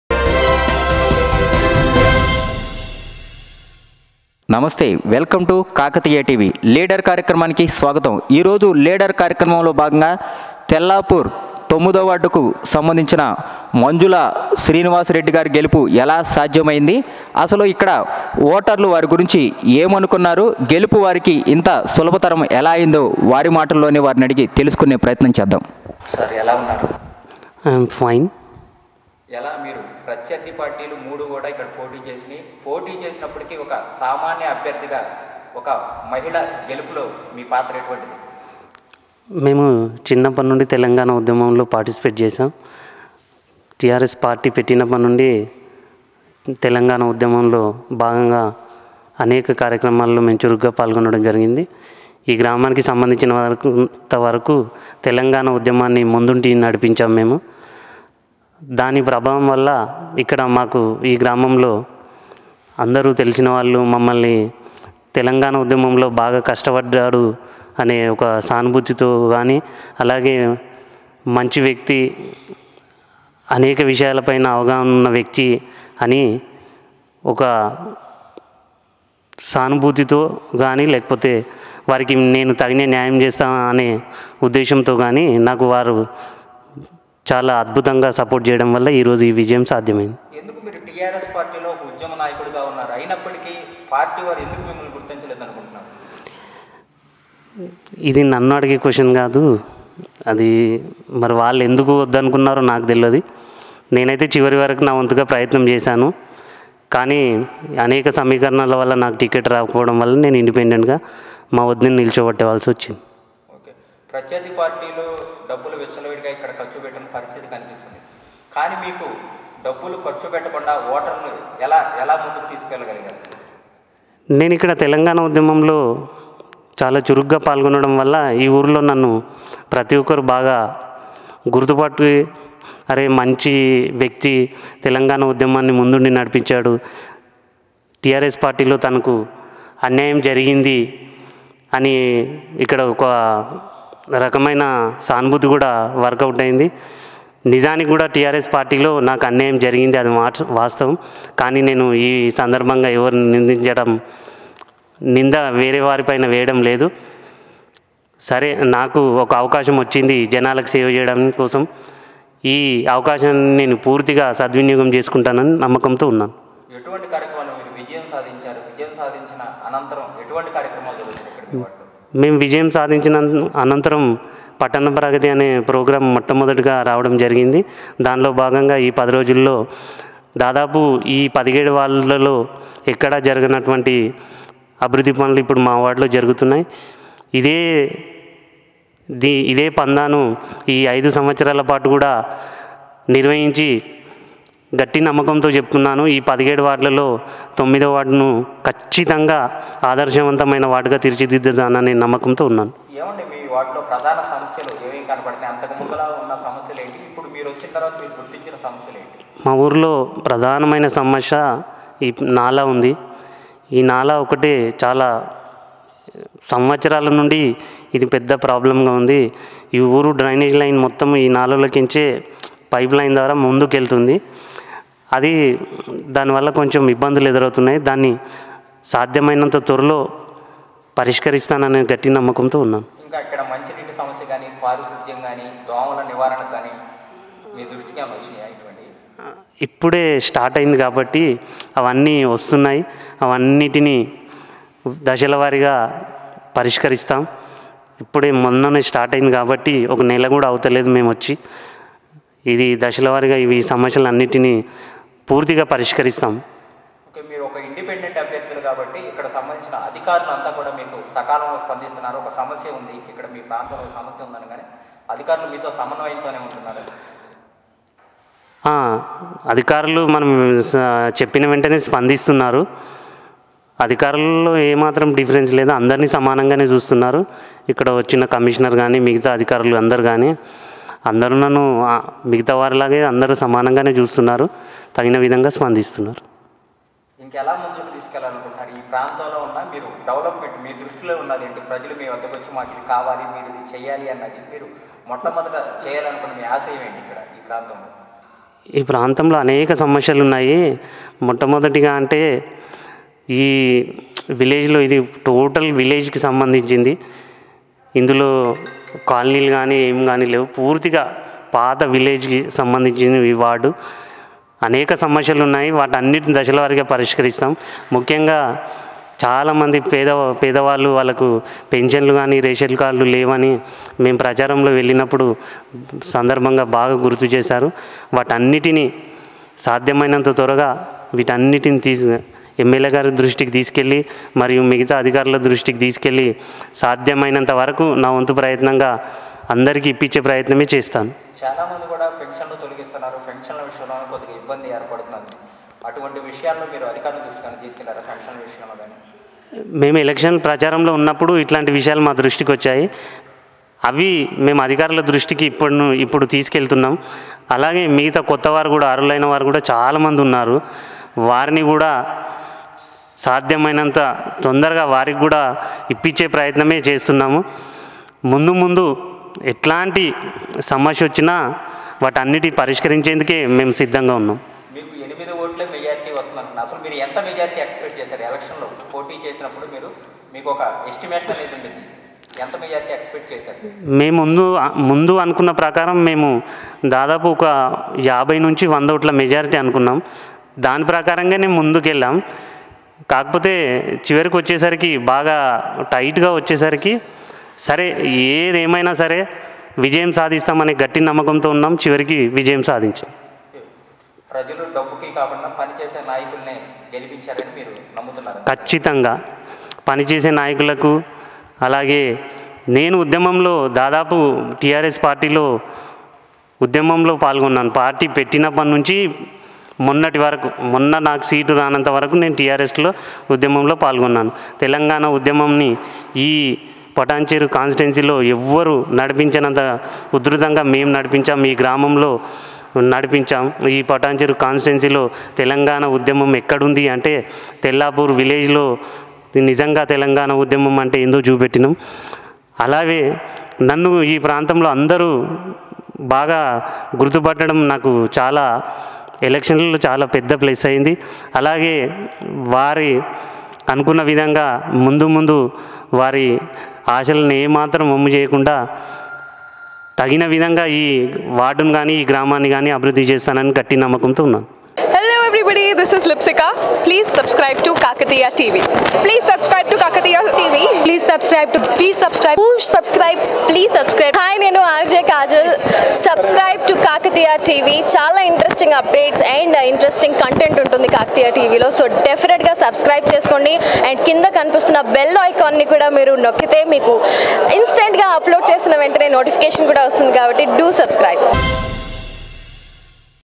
Tellapur Muncipality Councellor Manjula Srinivas Reddy Special Interview || KAKATIYA TV ll
Tellapur Muncipality Councellor Manjula Srinivas Reddy Special Interview l Telangana l Medak l Patancheru || KAKATIYA TV ll